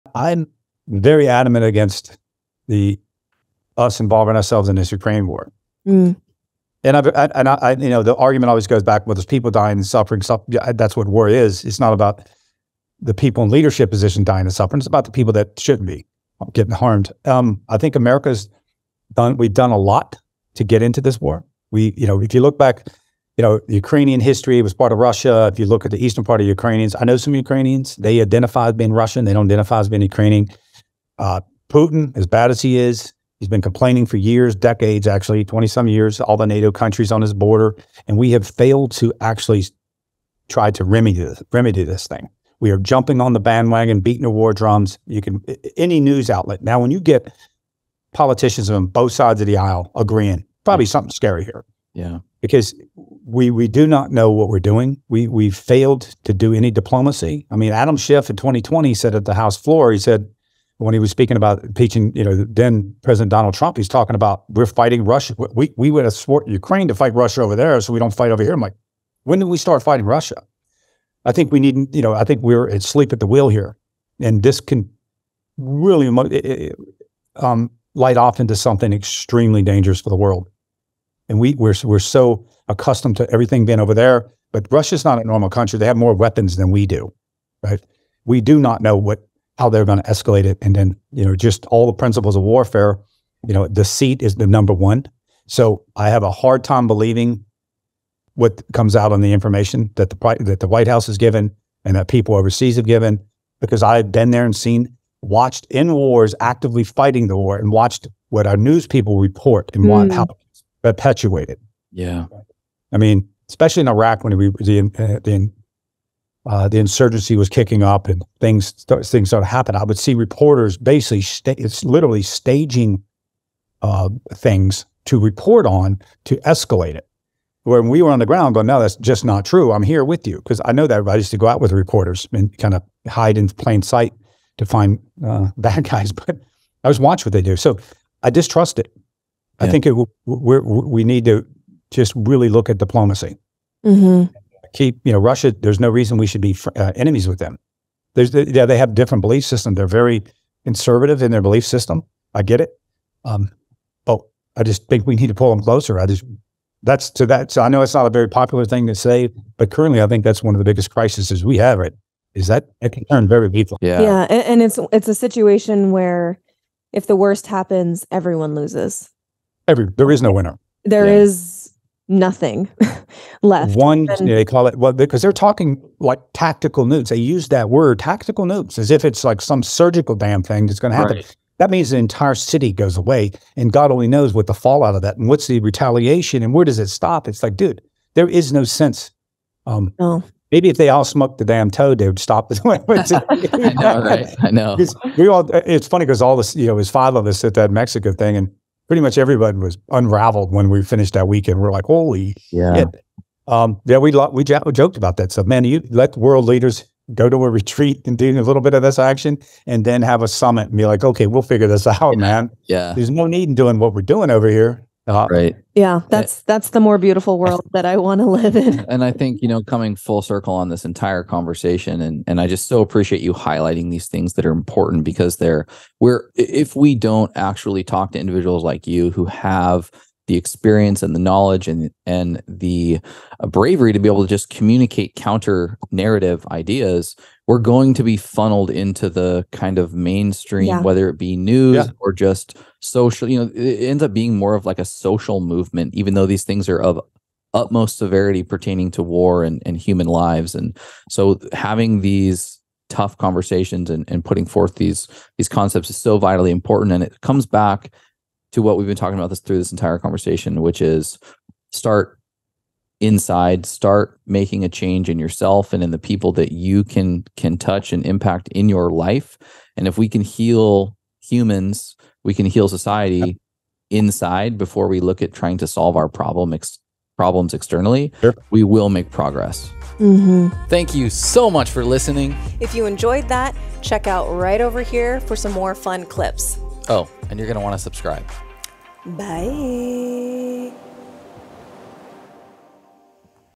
Retired NAVY SEAL Speaks Out Against America's Involvement in UKRAINE
Ed Hiner Navy SEAL commander, sharing some "unpopular opinions" in episode #185 of @themedicinpodcast